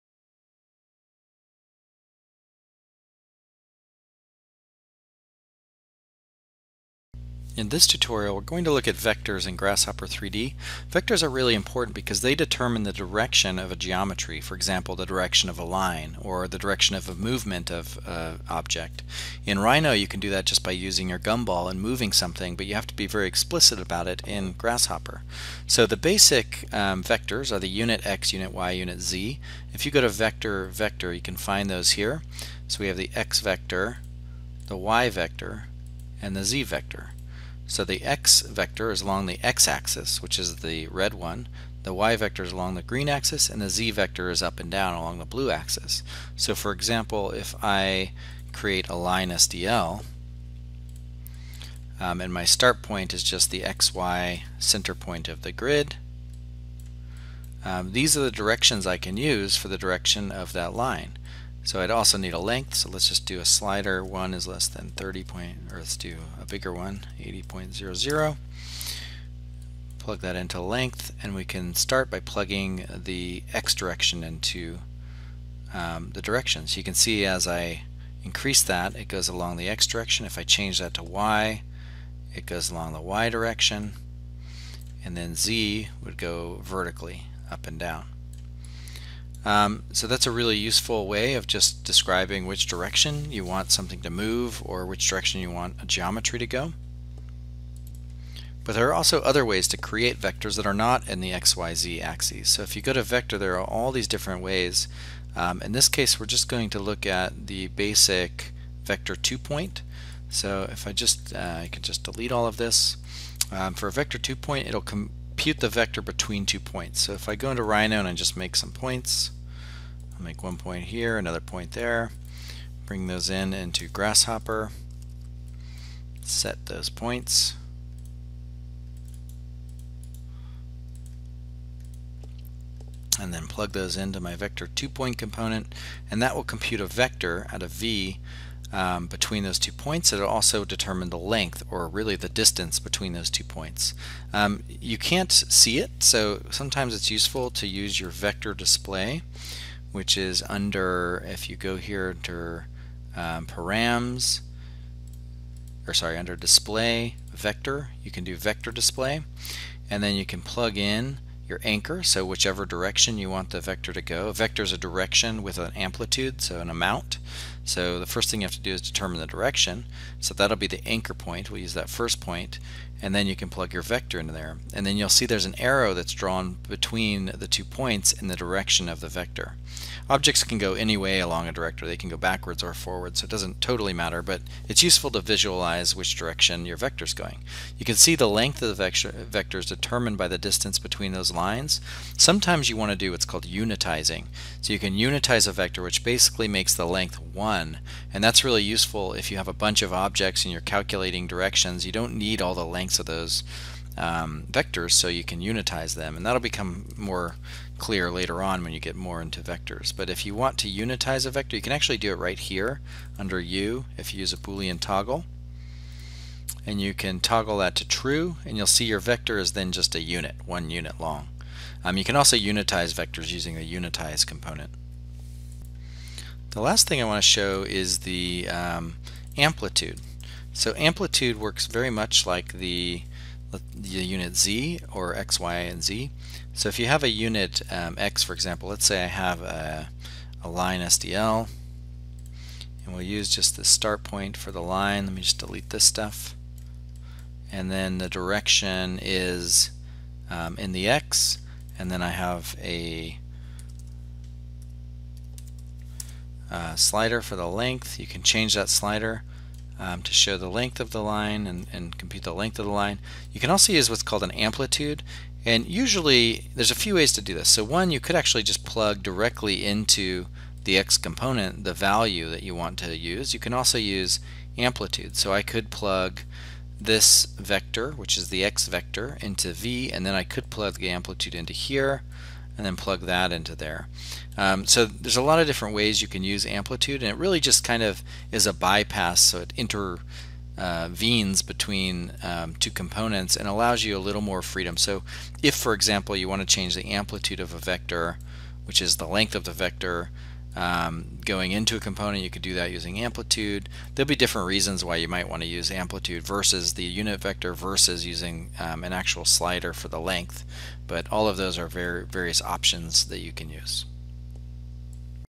Grasshopper Tutorial: Vectors
In this tutorial, I cover the basic Vector components in Grasshopper 3d.